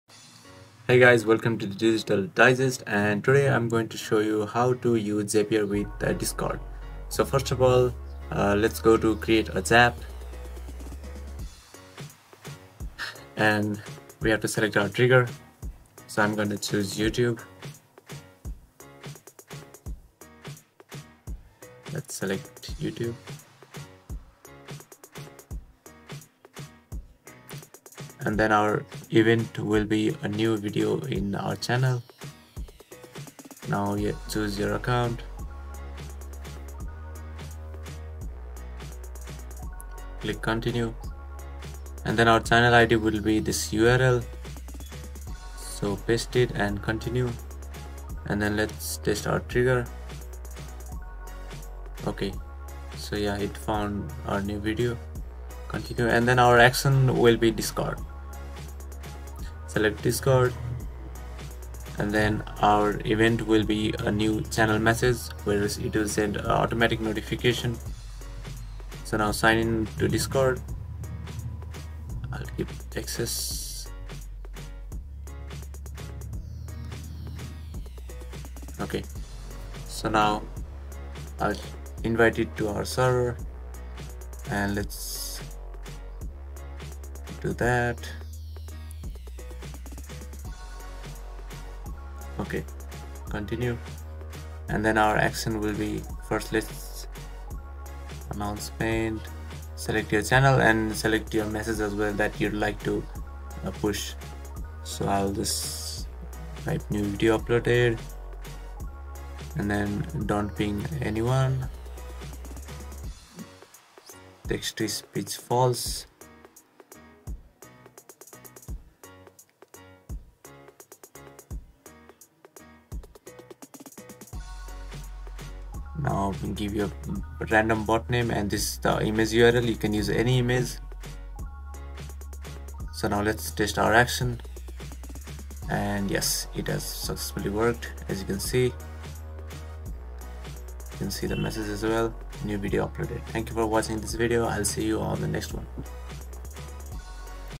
How to Use Zapier With Discord (Quick & Easy)
How to Use Zapier With Discord

In this video, I'm showing you how to use Zapier with Discord. Zapier is a platform that lets you connect different apps together, so you can do things like automate tasks and send messages between different Discord servers.

If you're looking to use Discord to connect with other Discord users, then this video is for you! By the end of this video, you'll be able to set up Zapier to automate tasks and send messages between different Discord servers. Don't miss out – watch it now!